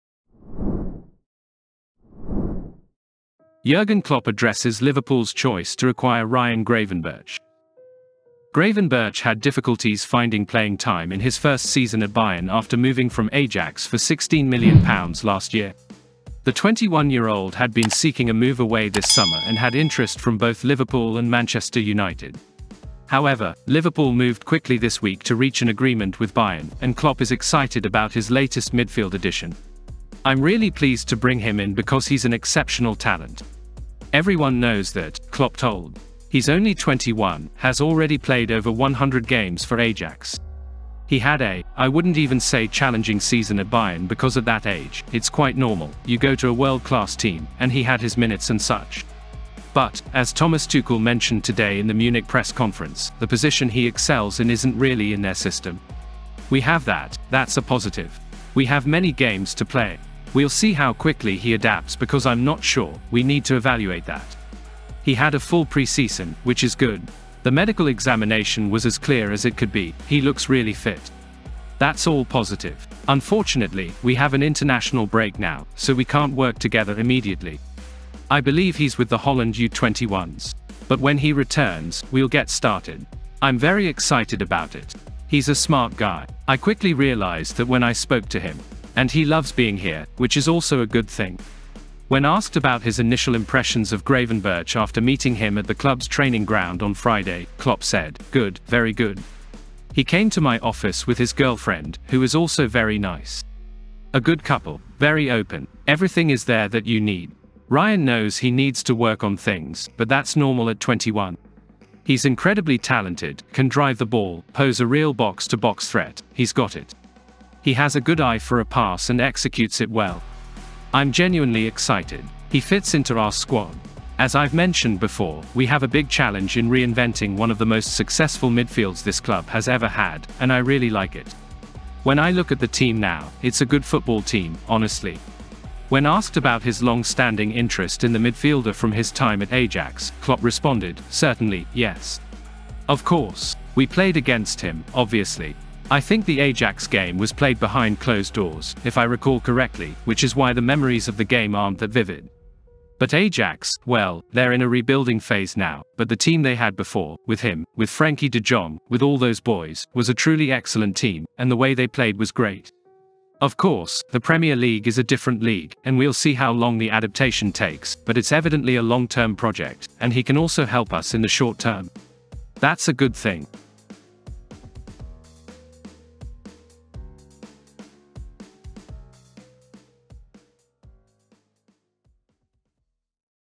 Jurgen Klopp Addresses Liverpool's Choice to Acquire Ryan Gravenberch.
Gravenberch had difficulties finding playing time in his first season at Bayern after moving from Ajax for €18.5 million (£16m) last year. The 21-year-old had been seeking a move away this summer and had interest from both Liverpool and Manchester United. However, Liverpool moved quickly this week to reach an agreement with Bayern, and Klopp is excited about his latest midfield addition.
"I'm really pleased to bring him in because he's an exceptional talent. Everyone knows that," Klopp told. "He's only 21, has already played over 100 games for Ajax. He had a, I wouldn't even say challenging season at Bayern because at that age, it's quite normal; you go to a world-class team, and he had his minutes and such. But, as Thomas Tuchel mentioned today in the Munich press conference, the position he excels in isn't really in their system. We have that – that's a positive.
"We have many games to play. We'll see how quickly he adapts because I'm not sure; we need to evaluate that. He had a full pre-season, which is good. The medical examination was as clear as it could be; he looks really fit. That's all positive. Unfortunately, we have an international break now, so we can't work together immediately. I believe he's with the Holland U21s. But when he returns, we'll get started. I'm very excited about it. He's a smart guy. I quickly realized that when I spoke to him. And he loves being here, which is also a good thing."
When asked about his initial impressions of Gravenberch after meeting him at the club's training ground on Friday, Klopp said: "Good, very good. He came to my office with his girlfriend, who is also very nice. A good couple. Very open. Everything is there that you need. Ryan knows he needs to work on things, but that's normal at 21. He's incredibly talented, can drive the ball, pose a real box-to-box threat; he's got it. He has a good eye for a pass and executes it well.
"I'm genuinely excited. He fits into our squad. As I've mentioned before, we have a big challenge in reinventing one of the most successful midfields this club has ever had, and I really like it. When I look at the team now, it's a good football team, honestly.
When asked about his long-standing interest in the midfielder from his time at Ajax, Klopp responded, "Certainly, yes. Of course. We played against him, obviously. I think the Ajax game was played behind closed doors, if I recall correctly, which is why the memories of the game aren't that vivid. But Ajax, well, they're in a rebuilding phase now, but the team they had before, with him, with [Frenkie] De Jong, with all those boys, was a truly excellent team, and the way they played was great.
"Of course, the Premier League is a different league, and we'll see how long the adaptation takes, but it's evidently a long-term project, and he can also help us in the short term. That's a good thing."